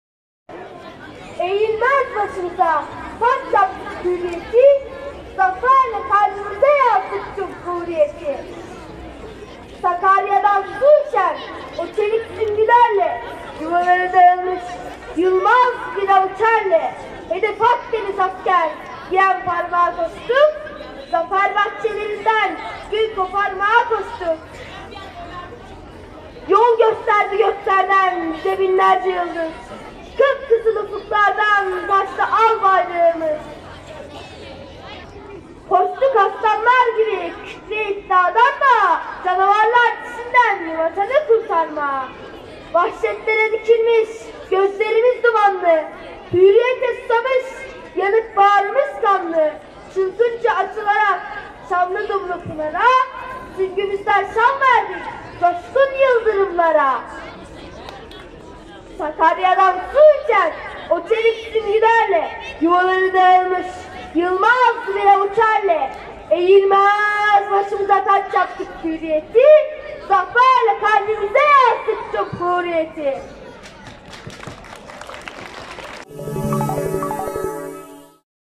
Çankırı İsmet İnönü Ortaokulunda Cumhuriyet coşkusu
Cumhuriyetimizin kuruluşunun 101’nci Yıldönümü Çankırı’da tüm okullarda coşkuyla kutlandı.